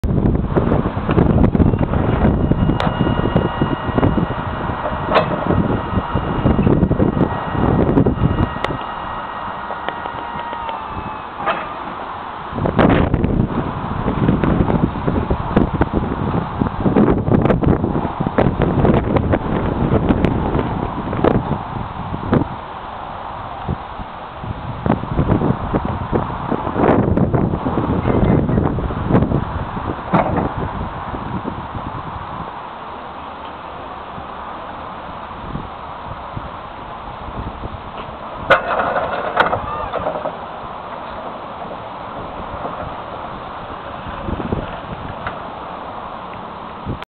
CAT EP16NT 3 WHEEL ELECTRIC FORKLIFT TRUCK
FORKLIFT TRUCK FOR SALE
3 WHEEL ELECTRIC COUNTERBALANCE
YEAR 2005
TRIPLE MAST
WHITE TYRES
48V BATTERY
CHARGER

£5,750 + VAT

FOR MOR EINFORMATION ON THIS FORKLIFT PLEASE GET IN TOUCH AND VISIT OUR